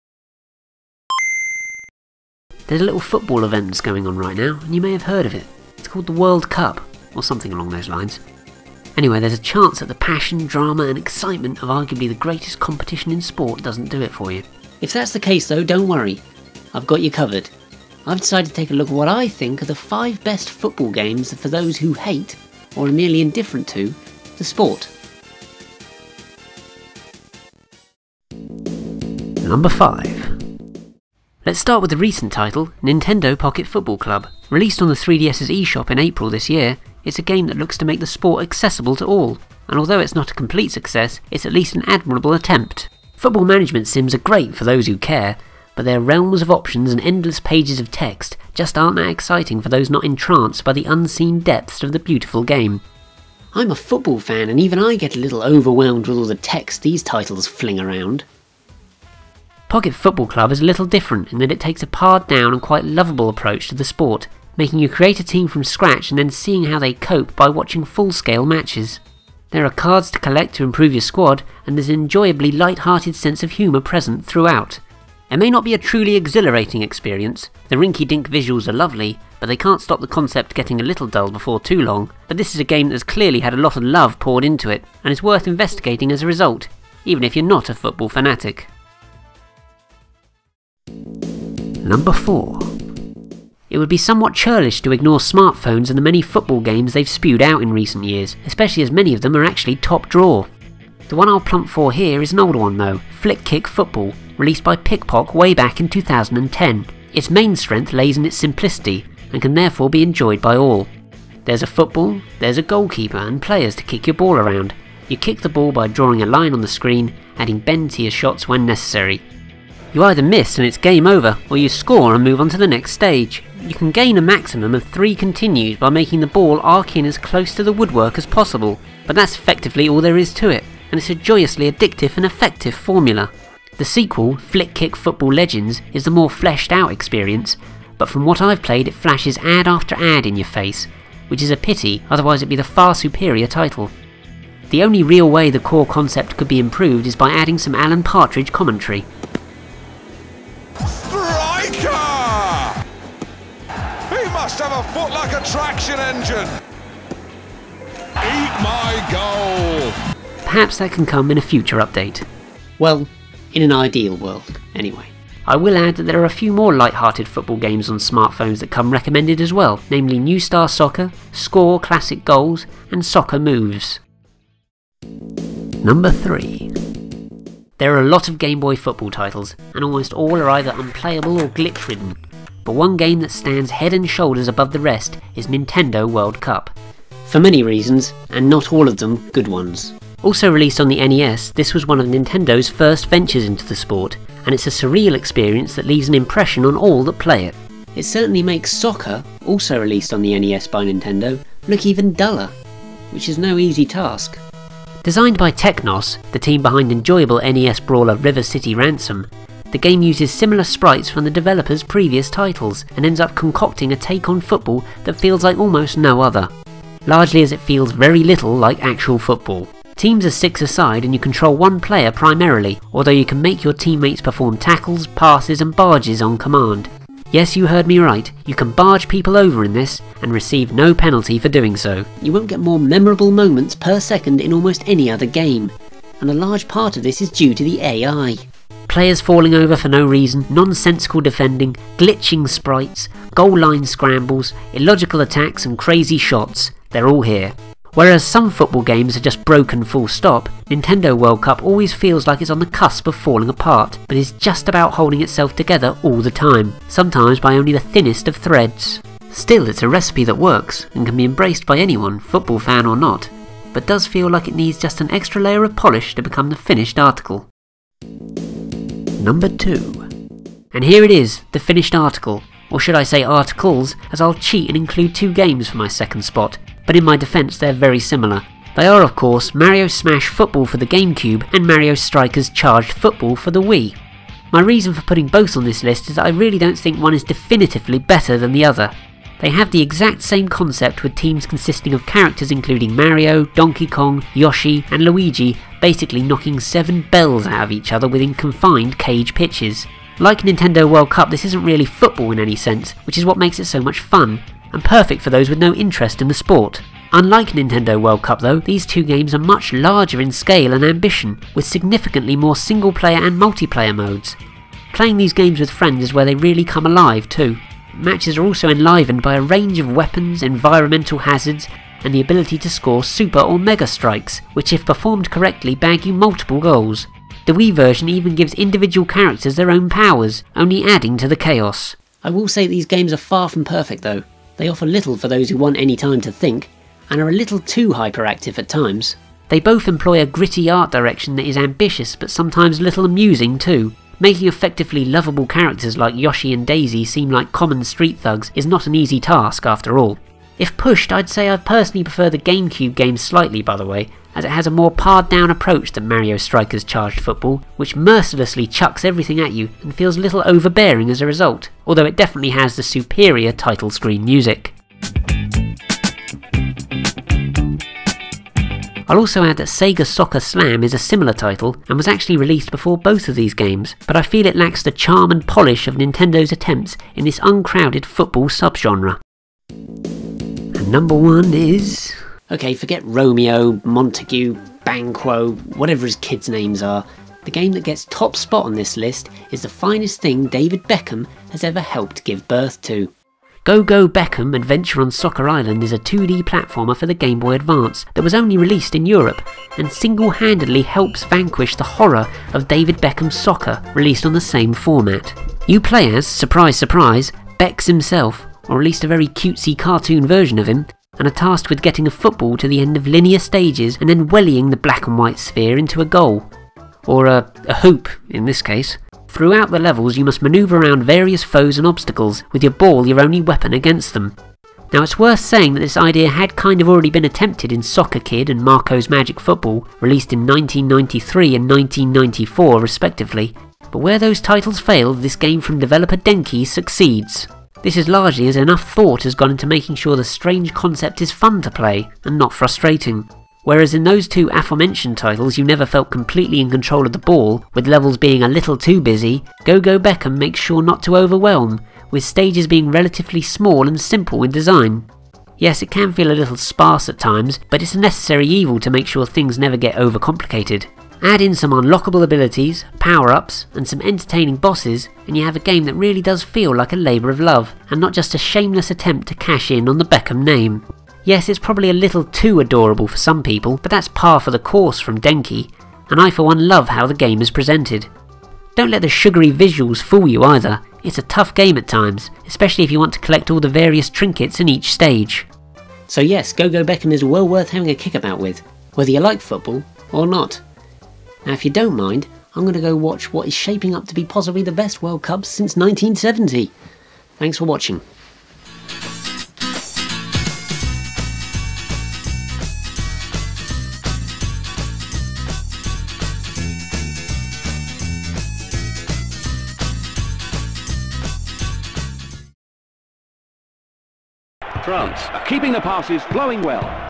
The 5 best football games for football haters
The World Cup is well underway - but there's a chance you don't give a monkey's about that. If that's the case though, this video is for you.

But if you do love football please do watch as well of course...

First person to name all 5 players used for the numbers wins a prize (maybe) by the way.

You can skip to each number using the links below too:

0:27 - Number 5
1:30 - Number 4
2:54 - Number 3
4:27 - Number 2
6:19 - Number 1